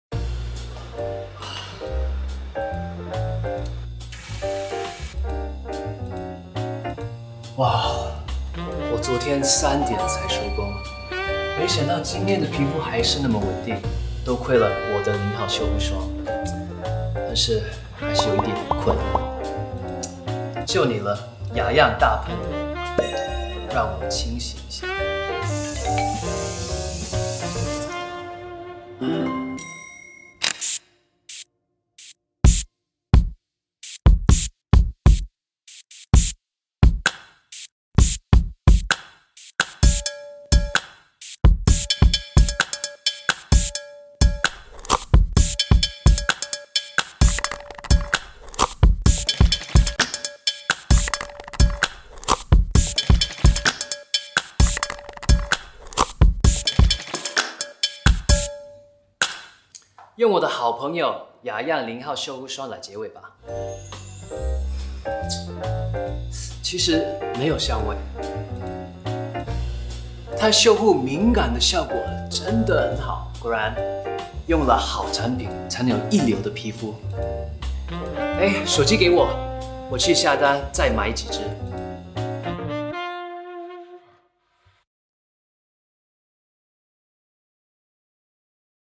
[Eng Sub] 20210902 Henry Lau 헨리 刘宪华​​ x @雅漾 Avène〜A day of Henry : Getting up chapter.
Cr : @雅漾 Avène  Weibo  update.